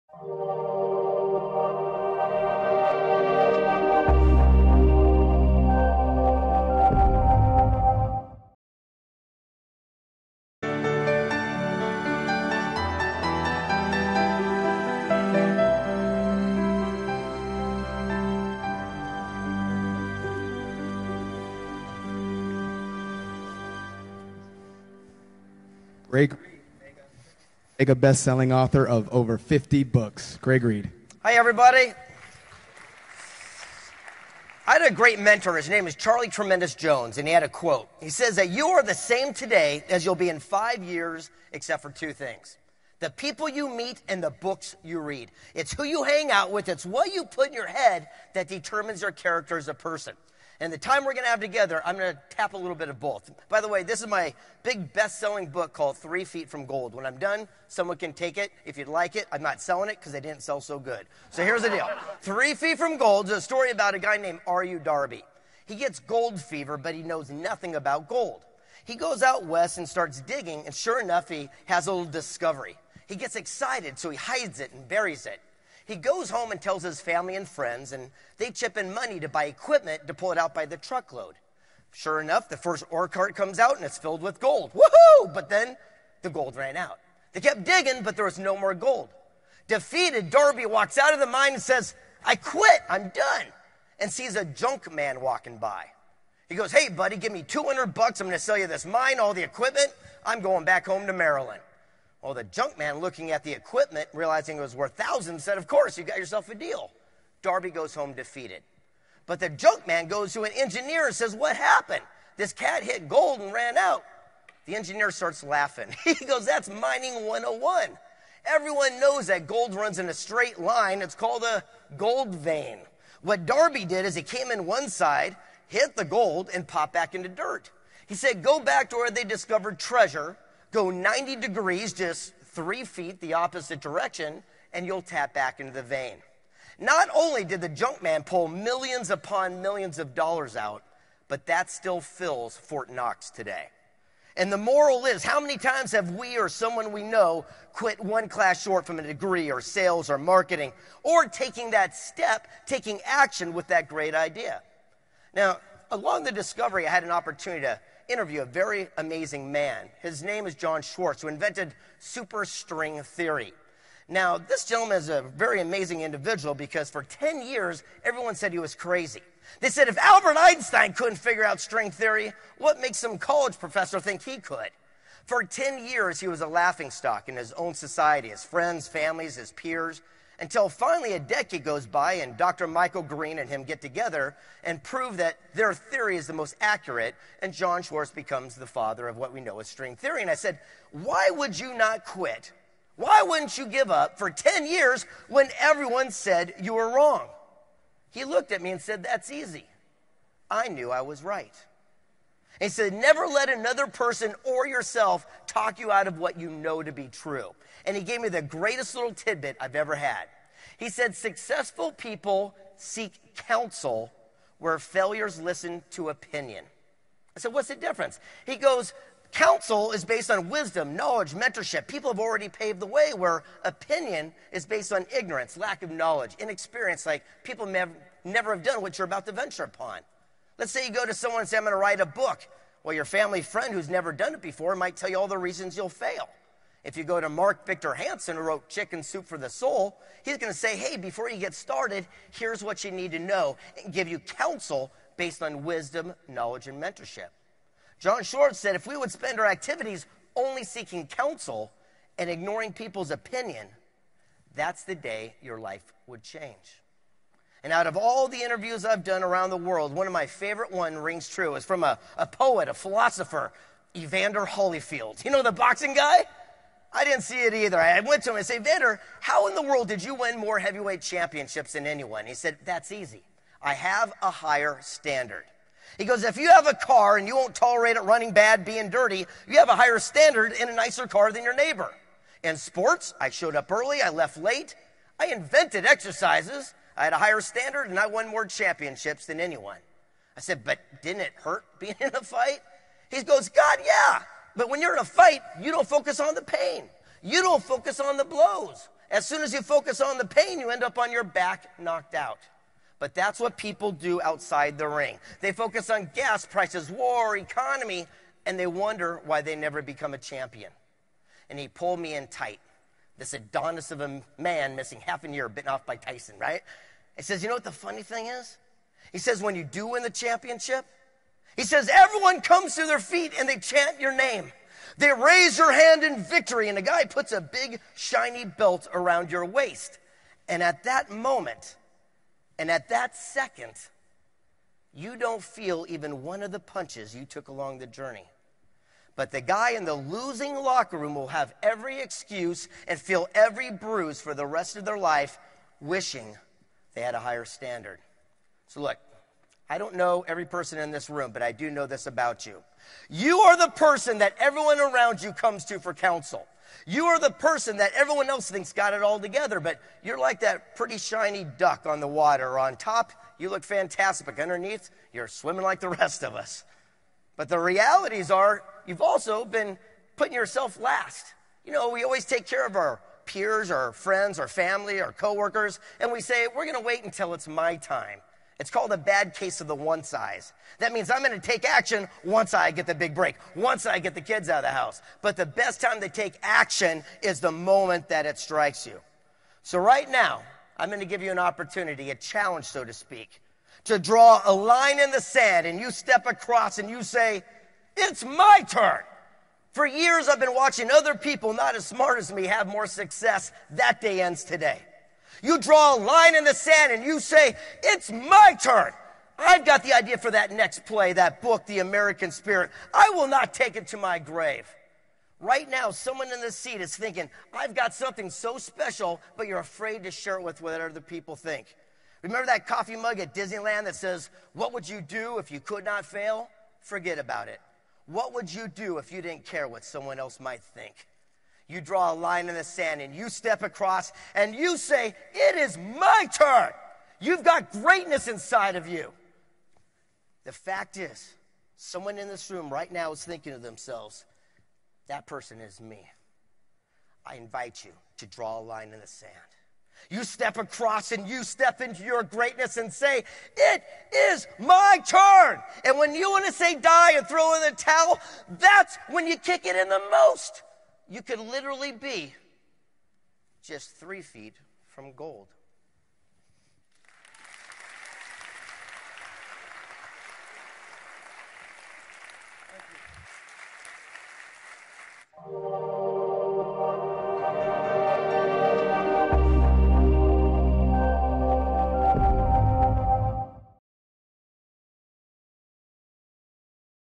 Greg Reid Novus Summit at The United Nations (Presentation)
Greg S. Reid presents at the United Nations NOVUS Summit 2016 – Science, Well-being and the Pursuit of Happiness  (July 11, 2016)

The NOVUS Summit aims to unite the science, technology and innovation community around the world and channel their brain power towards the much-needed actionable changes and scalable solutions

Greg S. Reid, Keynote Speaker was presented with the United Nations Global Goals Award Excellence in Leadership & Empowerment